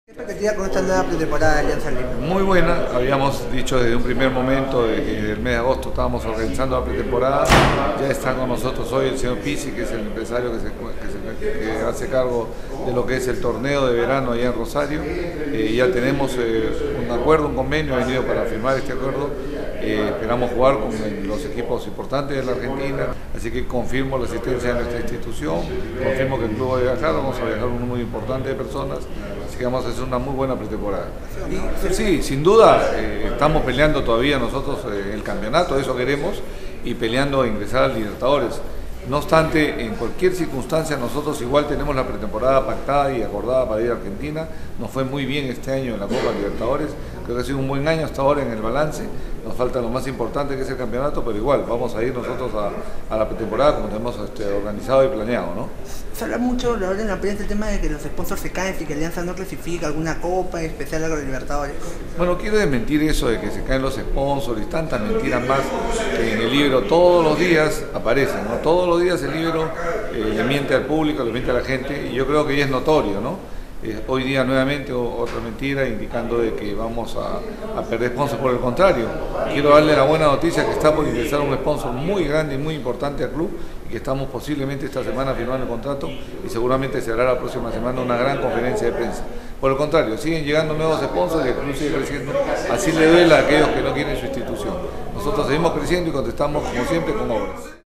El presidente blanquiazul anuncia la llegada de un nuevo y millonario sponsor al club.
Tras la prensa que anuncia la posible salida de los auspiciadores de club, si este no clasifica a la copa libertadores, Guillermo "Pocho" Alarcón desmiente esas versiones y anuncia la llegada de un nuevo y millonario sponsor a tienda íntima, este será presentado en los próximos días.